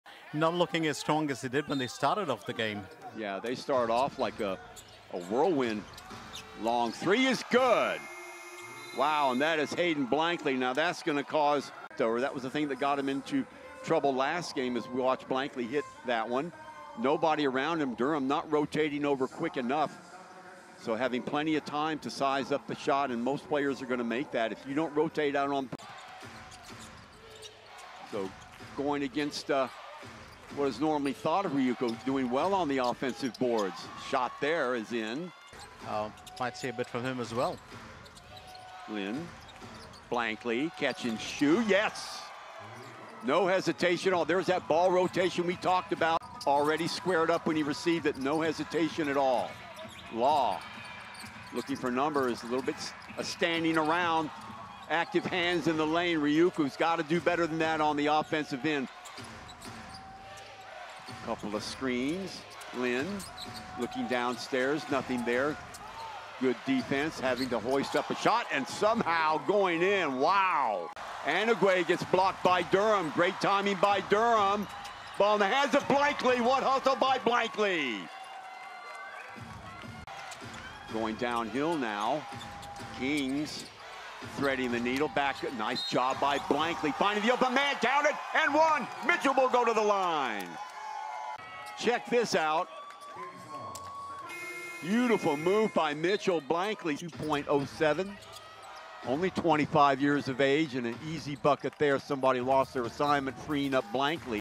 Game Highlights: New Taipei Kings Hayden Blankley 16 pts vs. Ryukyu Golden Kings | Jan 24, 2023
New Taipei Kings forward Hayden Blankley provided a spark off the bench against the Ryukyu Golden Kings, recording 16 points in a losing effort.